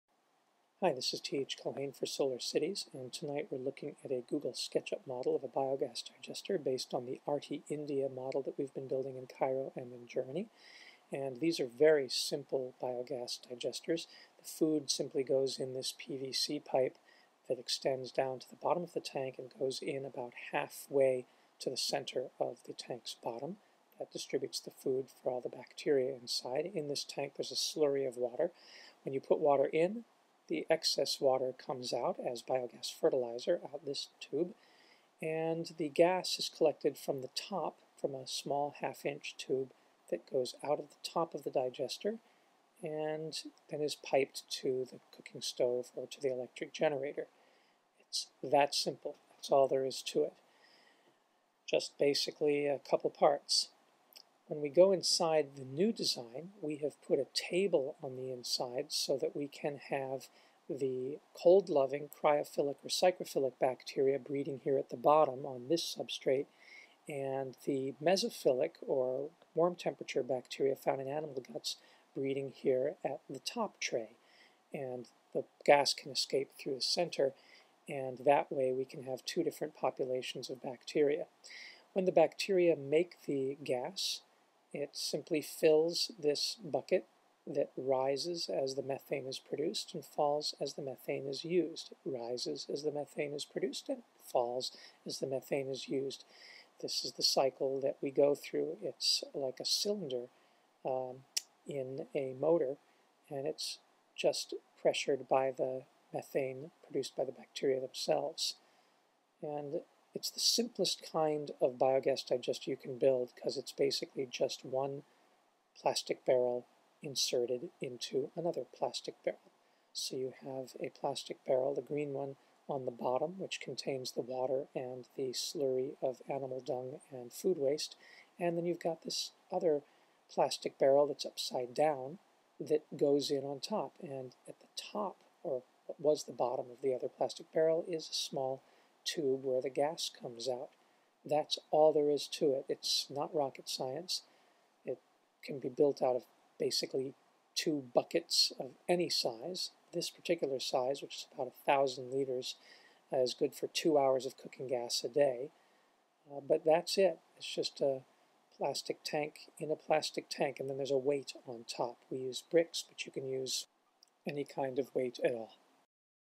Animation of a simple telescoping biogas digester
For this simple animation describing just how simple it is to build your own biogas digester I downloaded Google Sketchup 7 and then used Google 3D Warehouse to download a model of a digester by roilbilad130 called "Biogas untuk limbah dapur" which is Indonesian for "Biogas for Kitchen Garbage".  I changed the food inlet pipe and the fertilizer output pipe to reflect the way they work in the ARTI India systems we have been building in Cairo and Germany and set different sequence animation key frames in Sketchup and did screen capture in Snap Z Pro.  Building real digester is about as easy as building a digital one in sketchup -- it really is just two plastic barrels, one inverted inside the other, with three pipes -- one to get the food in, one to take the liquid fertilizer out and one at the top to deliver the gas to your cookstove or generator. 1000 liter tanks should get you about 2 hours of cooking gas a day if you live in a warm climate.  As shown, we intend to use psychrophilic bacteria at the bottom to increase cold weather production.
The process is very simple -- on the first day you put in about 40 kg of animal manure (horse or cow work fastest, but anything will work) and fill up the container with water.  Then place the gas collector on top with the valve open so it sinks down into the bottom container, driving out the air. Then close the valve. After about 3 weeks the gas being created inside the container will be flammable. From that point on you start adding kitchen waste ground up with water in a blender (be careful not to burn up your blender!)  Because methane is lighter than air, if the valve in the top container is closed it will not escape (think of a diving bell or a hot air ballon -- they are open on the bottom and no gas escapes).  Even though there is open space around the sides the gas is trapped in the top container until you open the valve to bring it to the kitchen. Hope that helps!